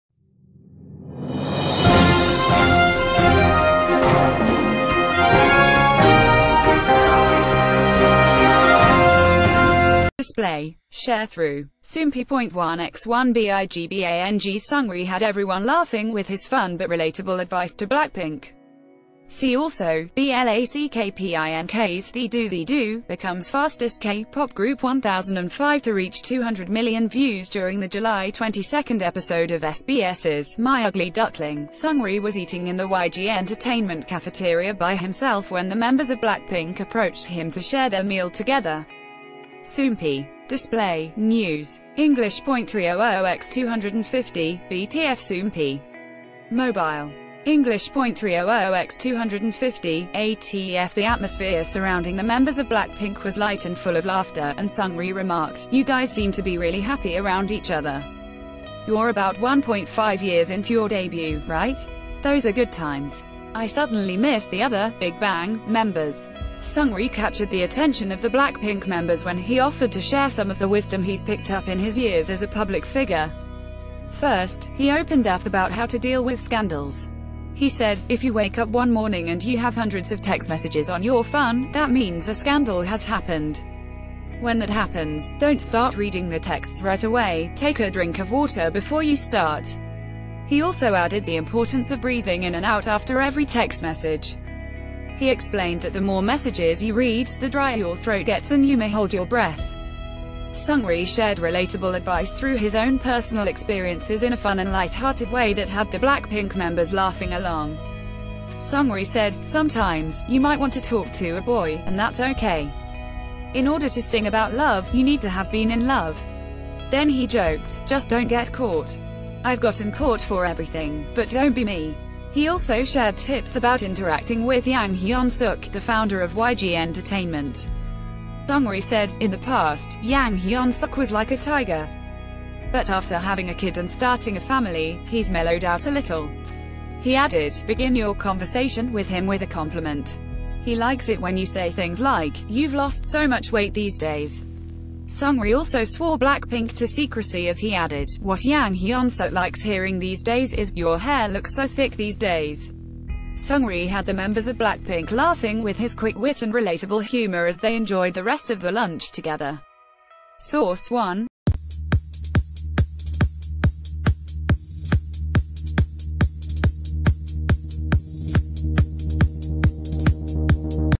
BIGBANG's Seungri Gives BLACKPINK Advice On How To Approach Scandals And Yang Hyun Suk
Display. Sharethrough. Soompi.1x1 BIGBANG’s Seungri had everyone laughing with his fun but relatable...